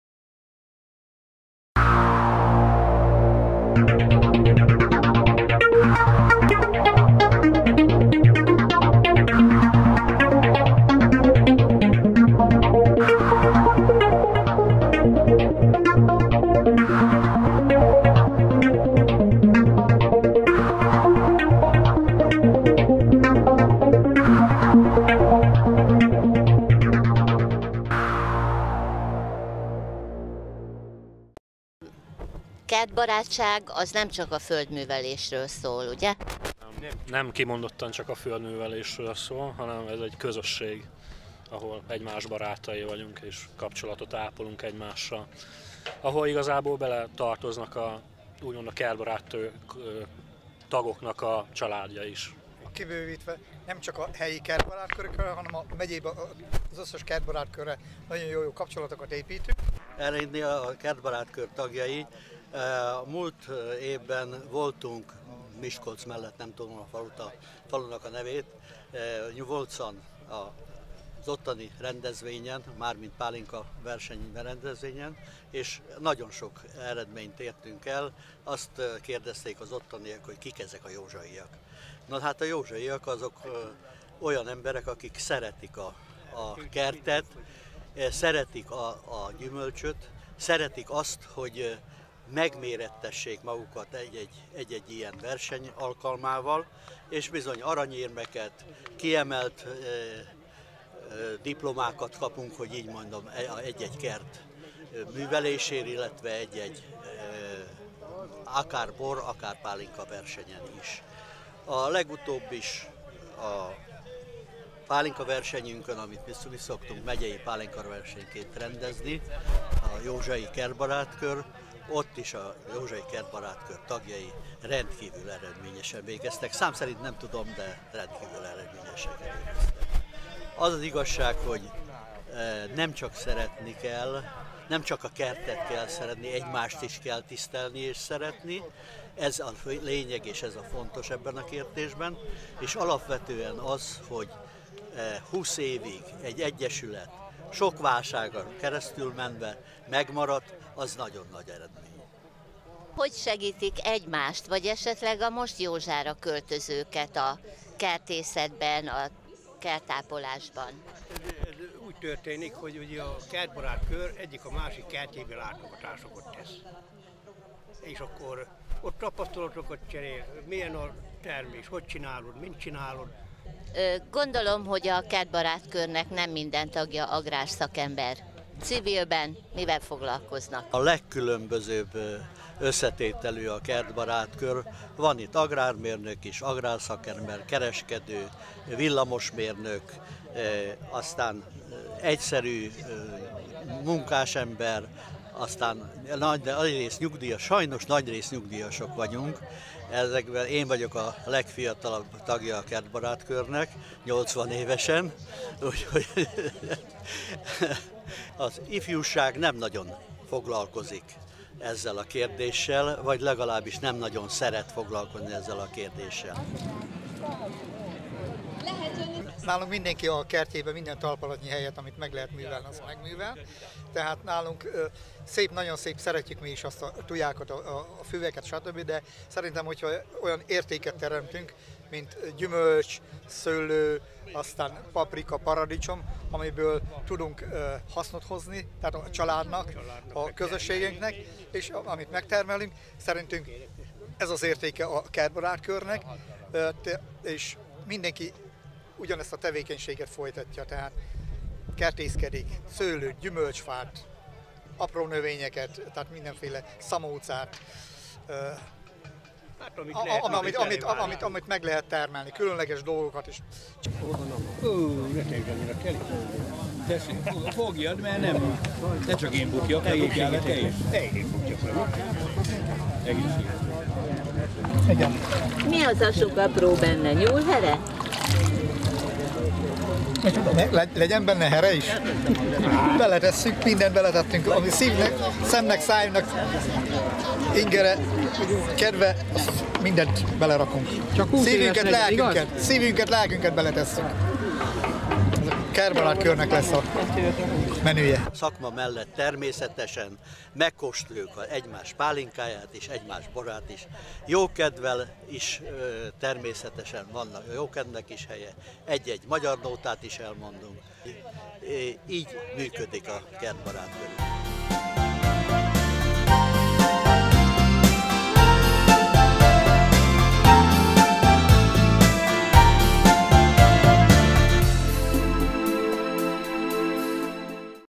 Kertbarát kör - Kultfilm 6
A Józsai Kertbarát Kör, amellett, hogy igen komoly tevékenységet folytat, -kicserélik tapasztalataikat, segítik egymást - igazi közösséggé csiszolódtak, akik a munkán kívül szabadidejükben is jól érzik egymással magukat.

Készítette: Varga József